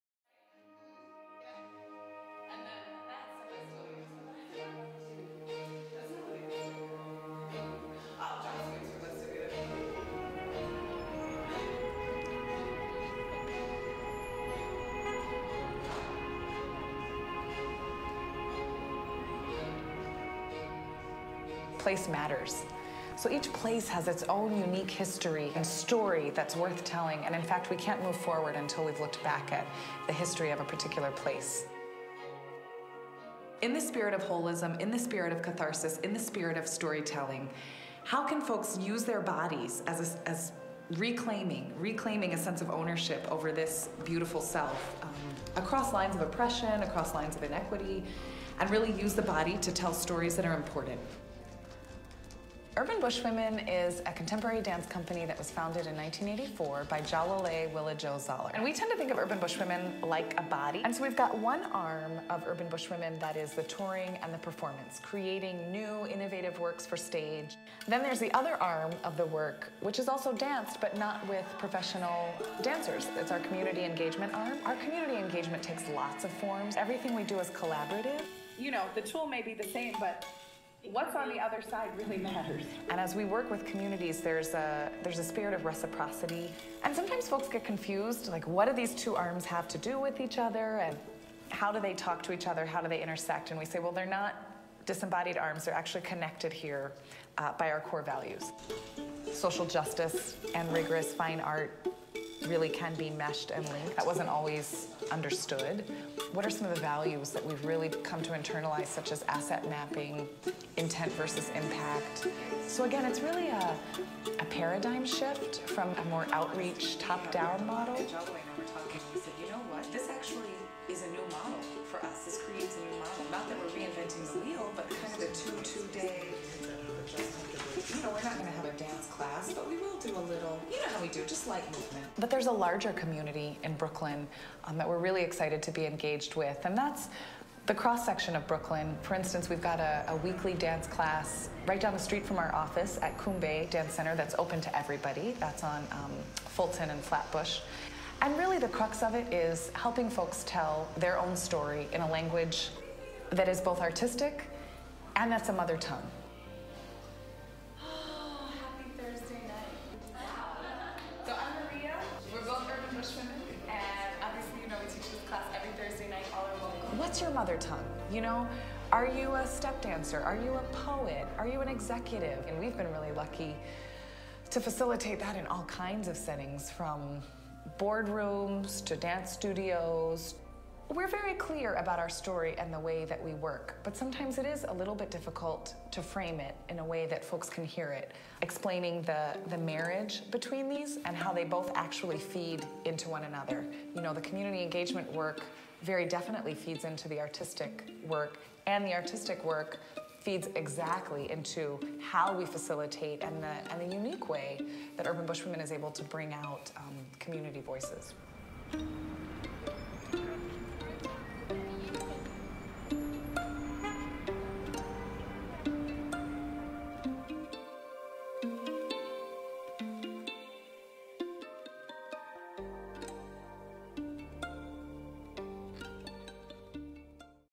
UBW BOLD: A Behind-The-Scenes-Look
Take a behind-the-scenes look at Urban Bush Women's BOLD initiative (Builders Organizers and Leaders through Dance) and how our dance company works! This video was filmed for Leveraging Investments in Creativity (LINC), which was a ten-year national initiative to improve the conditions for artists working in all disciplines.

Video by Cassim Shepard.